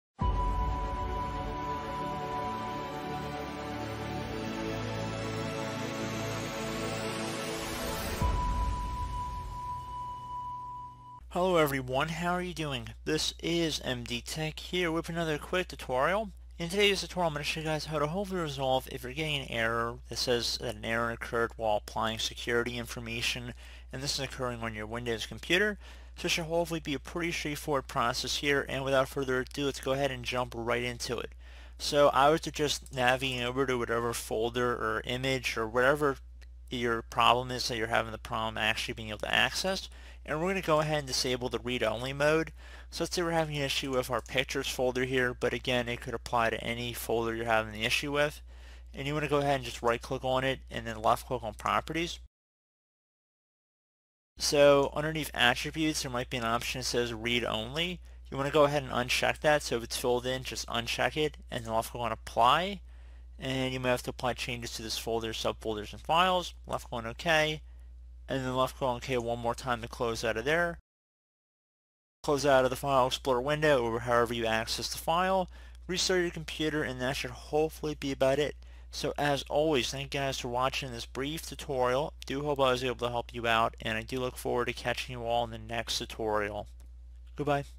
Correção: Ocorreu um erro ao aplicar informações de segurança
Ocorreu um erro ao aplicar as informações de segurança CORREÇÃO [Tutorial].

Esse erro geralmente aparece ao tentar alterar várias configurações de segurança de um arquivo ou pasta localizada em seu computador ou em uma mídia externa conectada a ele. A mensagem de erro geralmente é precedida por uma mensagem de 'acesso negado' que aparece ao tentar mover, copiar ou excluir os arquivos aos quais você não tem acesso adequado.

Este tutorial se aplica a computadores, laptops, desktops e tablets que executam os sistemas operacionais Windows 11, Windows 10, Windows 8/8.1, Windows 7. Funciona para todos os principais fabricantes de computadores (Dell, HP, Acer, Asus, Toshiba, Lenovo, Samsung).